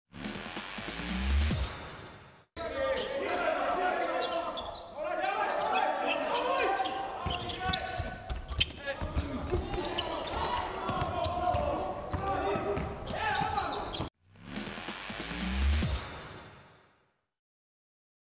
Will Mosley says no! (Krka - Partizan NIS, 19.12.2020)
Will Mosley of Partizan NIS came up with this block in the game against Partizan NIS at home in Leon Štukelj Sports Hall, Novo mesto. Check it out!